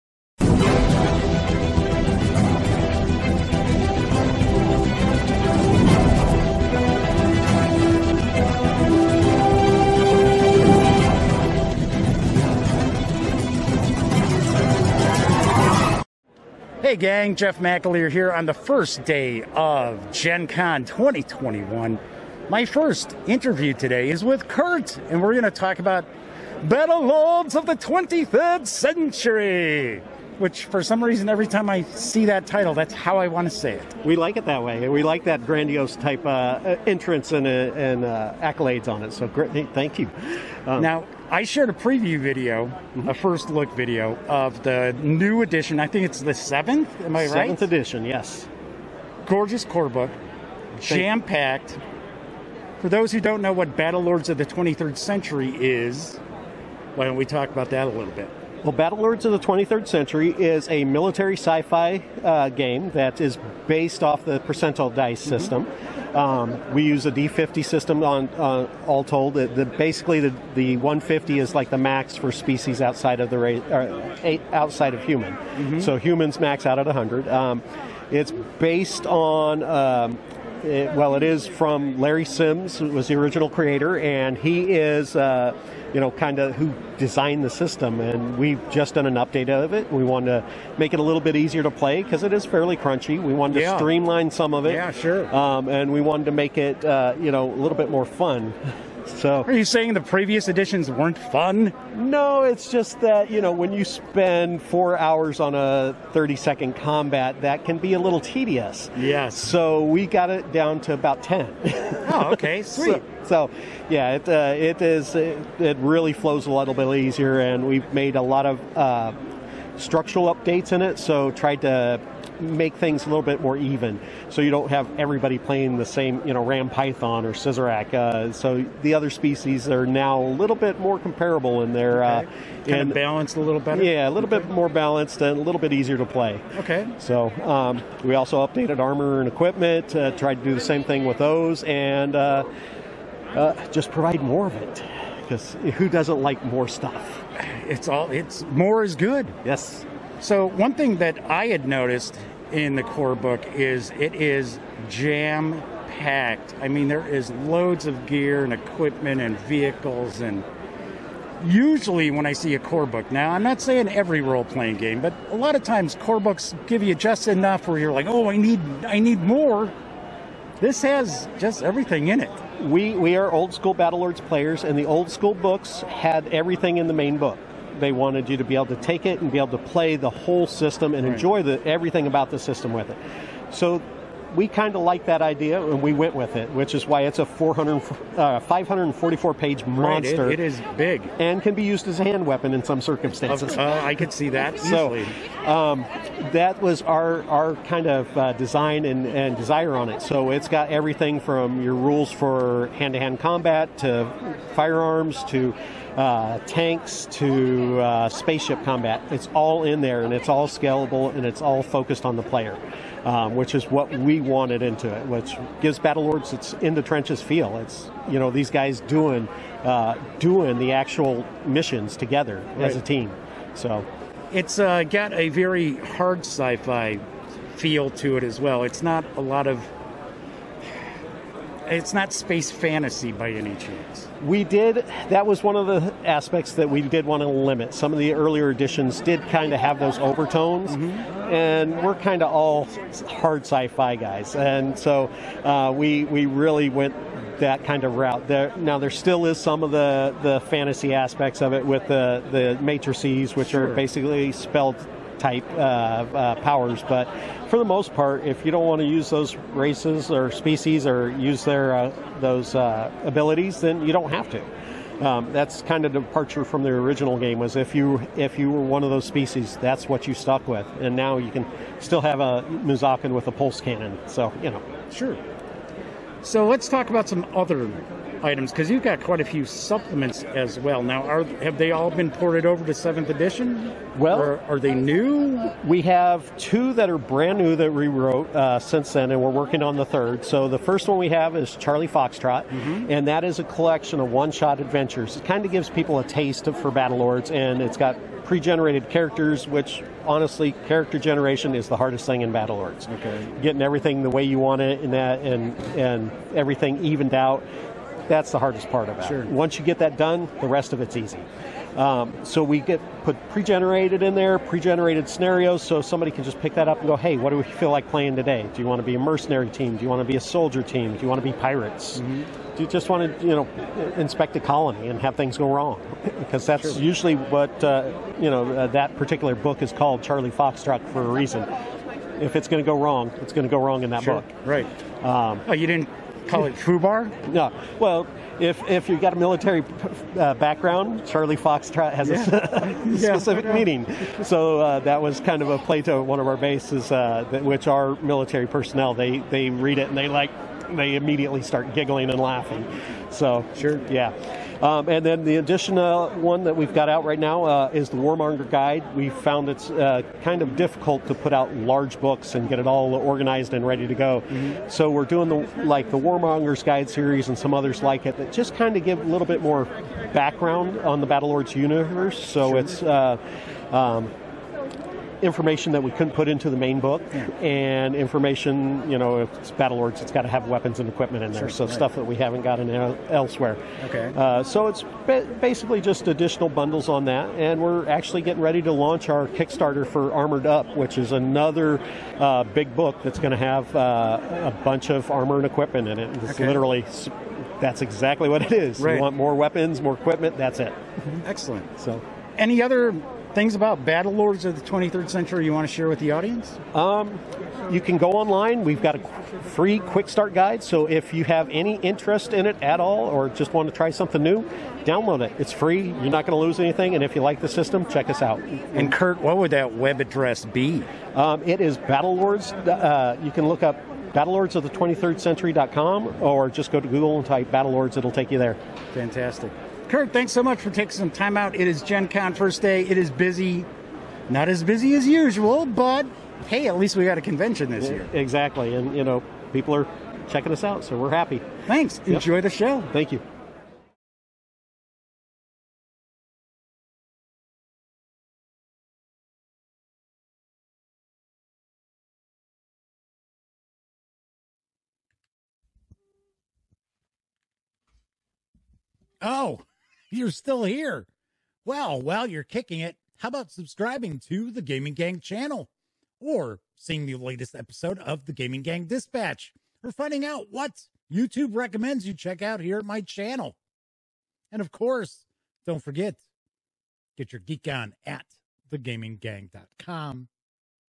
Battlelords of the 23rd Century at Gen Con 2021
Jeff meets with Kurt Willis to talk about the latest edition of the military science fiction roleplaying game Battlelords of the 23rd Century. Check out the first look video of the corebook right

Do you enjoy The Gaming Gang videos and website? Then how about buying Jeff a cup of coffee